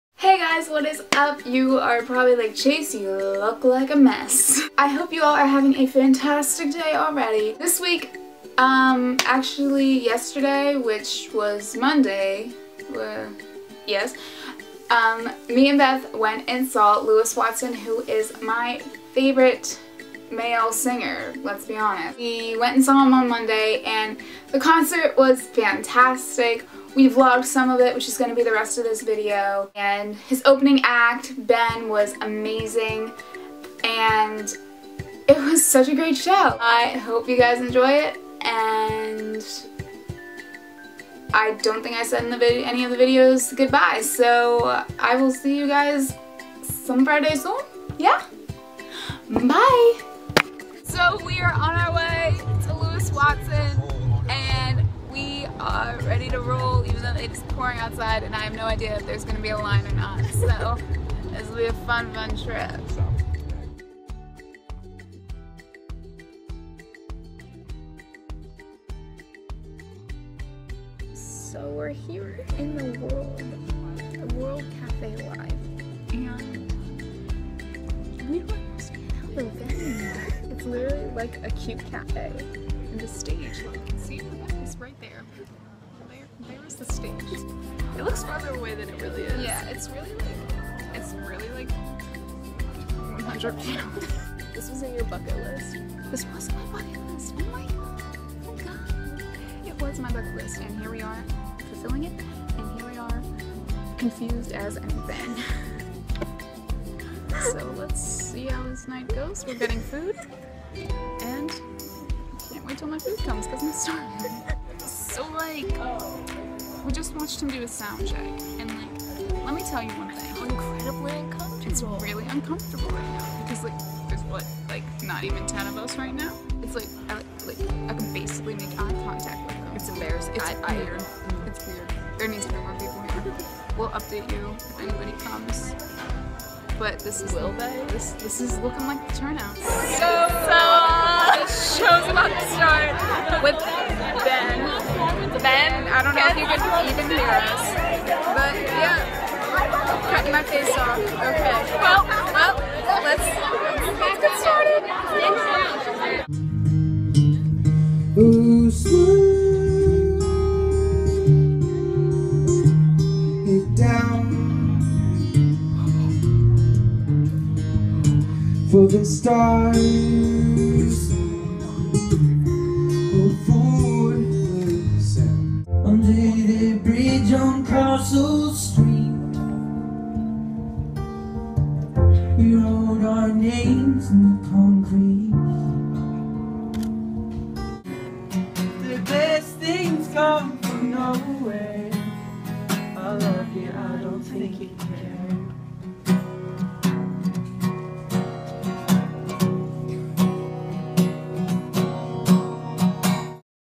LEWIS WATSON CONCERT // VLOG
happy friday this week we go see lewis watson in concert and are amazed at his wonderful talent

LINKS

Vine: BethxChase